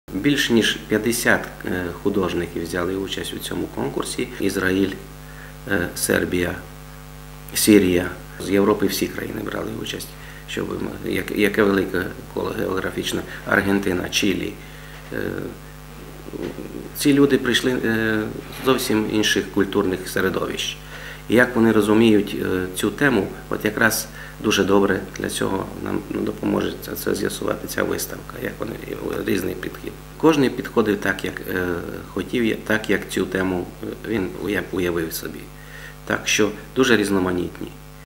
Тиса-1.Злободенна, сатирична, влучна.
Карикатура -- специфічний художній жанр, що в гумористично-сатиричній формі зображає важливі соціальні й громадсько-політичні явища, побутові ситуації, а також реальні постаті чи характерні типажі. Передати лаконічними засобами глибокий зміст удається обраним і тоді   карикатурні твори здатні здолати кордони. "Чхання мозку" - під такою назвою в Ужгороді відкрили Міжнародну виставку карикатур знаних майстрів із більш, як десяти країн.